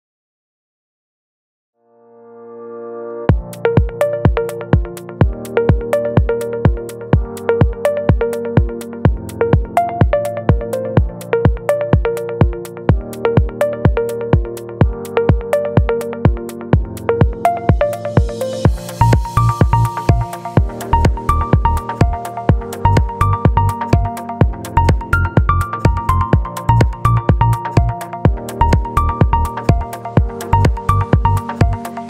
野球上達のための切替動作改善トレーニング！ ハードル11
ライバルと差がつく！野球上達のための走動作改善！最強フットワークトレーニング！
ミニハードル基礎編　ハードル11
『スキップ→スプリント』

この動画で使用している商品↓
スピードバウンスバックハードル